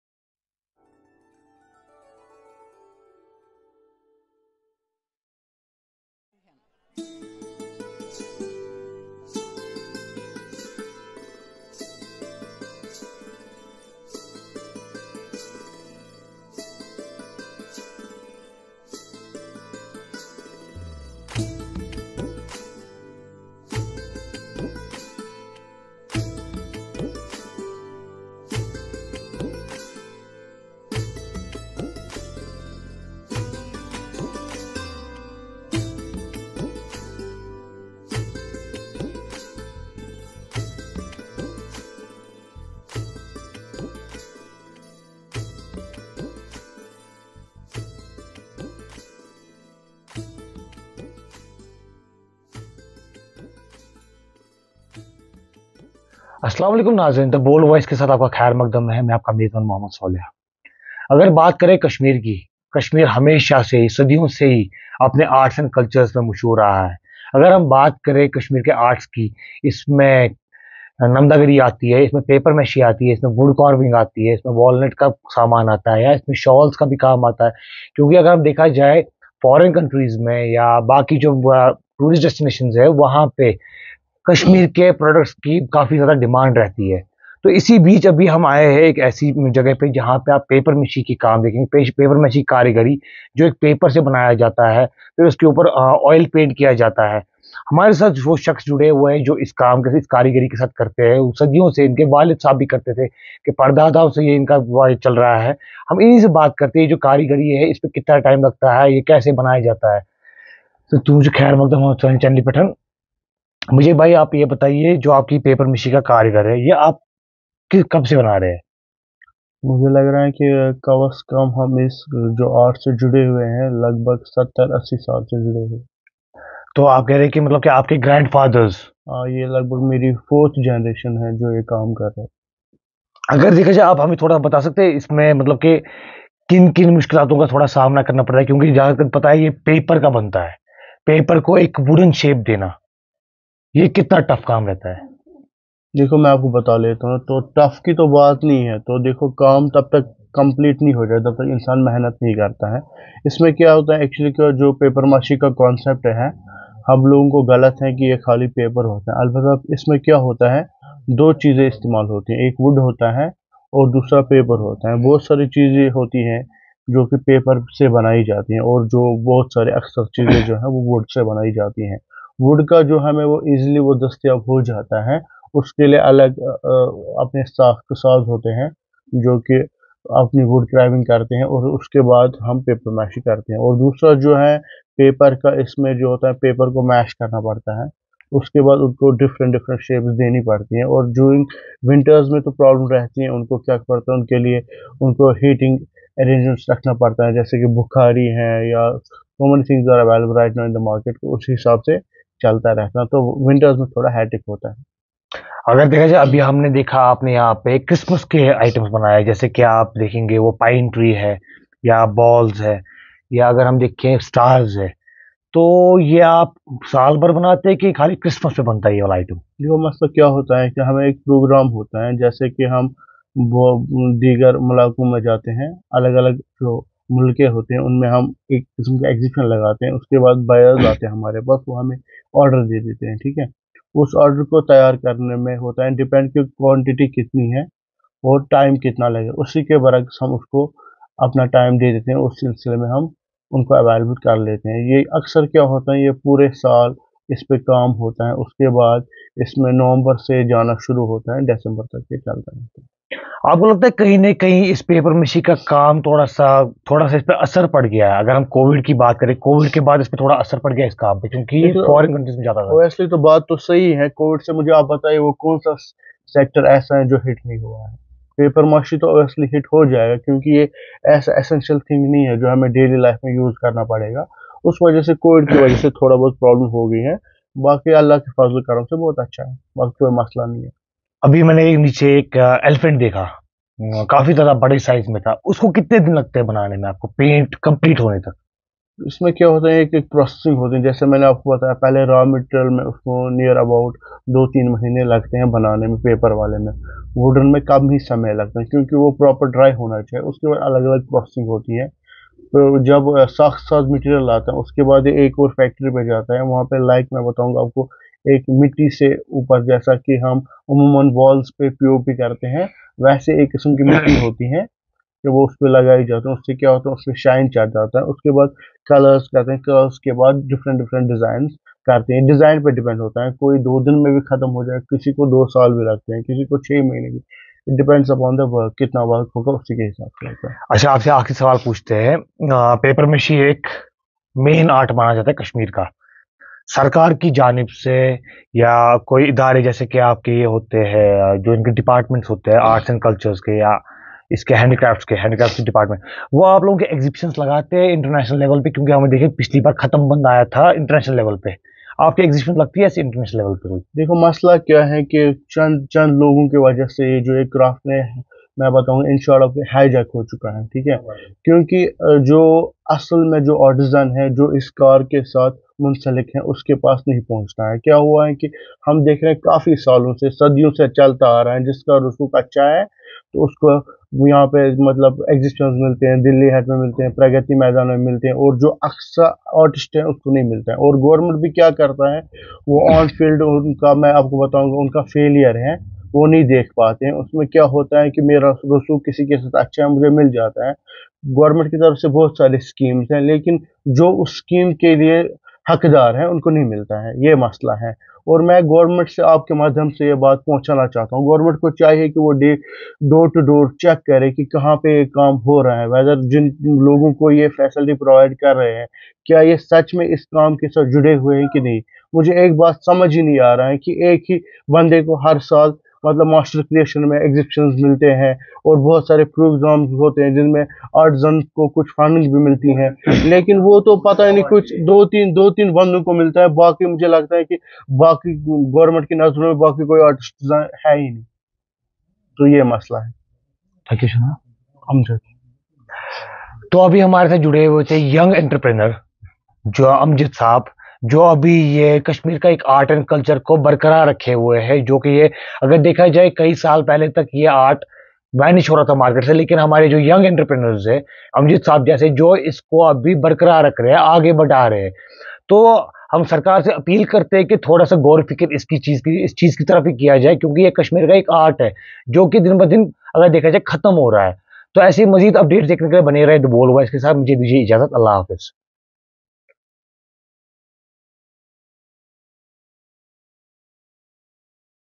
Meet Amjad Bhat an MBA who safeguards the endangered Kashmiri art of Paper Mache.
MBA degree holder Amjad Bhat safeguards the endangered Kashmiri art of Paper Mache, preserving its legacy from the brink of extinction
Reported Mohammad Sauleh